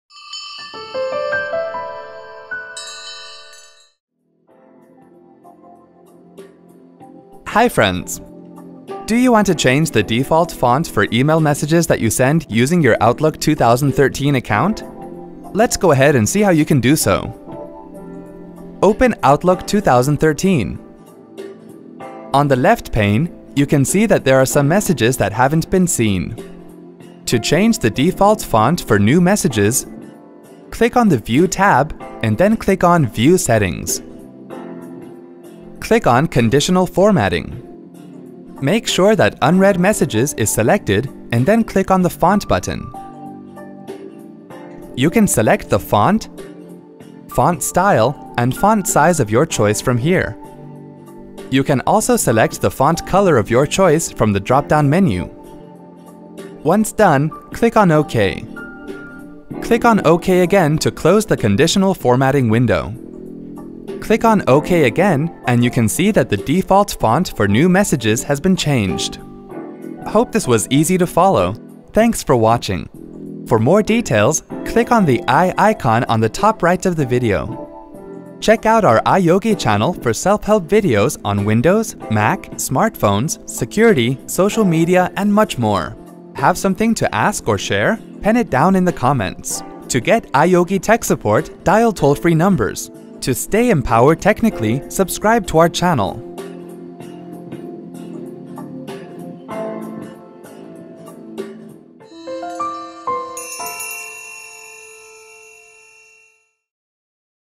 How to change default font for new messages in Microsoft® Outlook 2013 :Tutorial
Do you want to change the default font for e-mail messages that you send using your Microsoft® Outlook 2013 account? Watch this video and follow the steps to change default font for new messages in Microsoft® Outlook 2013.